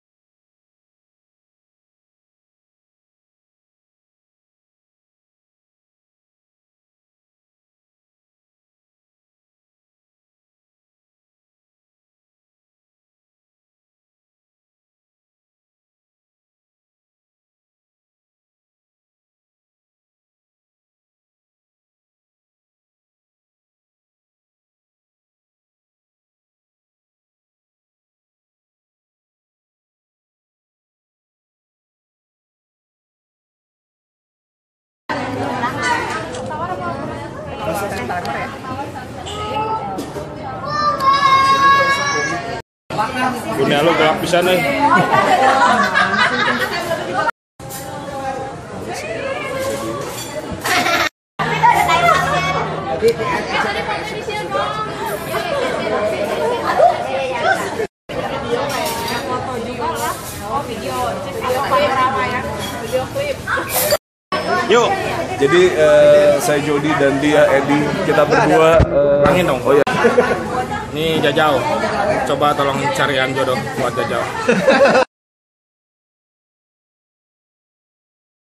Temu Kangen St.Angela Class of 1997 Compilation
Sedikit kenangan-kenangan dari acara Temu Kangen Part.1 (Bandung, 7 Febuari 2015)